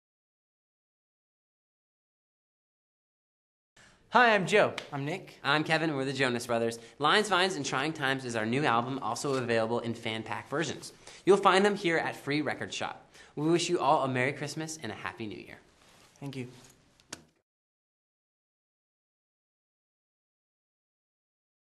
Free Record Shop Kerstboodschap - Jonas Brothers
Een kerstboodschap van Andrei Lugovski, exclusief voor Free Record Shop!